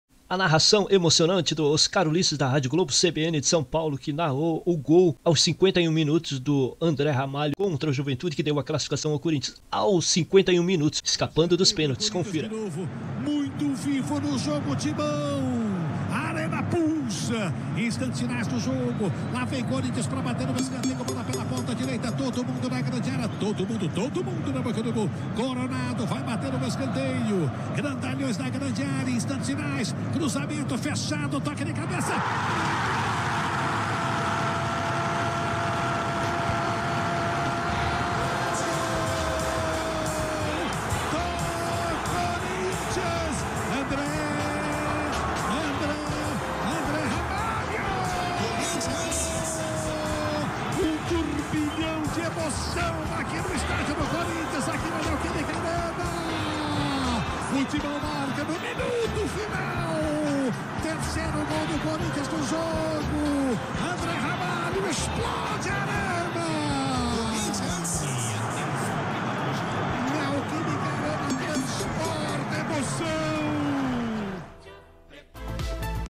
Narração emocionante da rádio Globo e CBN de SP gol de André Ramalho que deu classificação do Timão
Créditos ao vídeo Sportv e rádio CBN e Globo São Paulo
Narração de Oscar Ulisses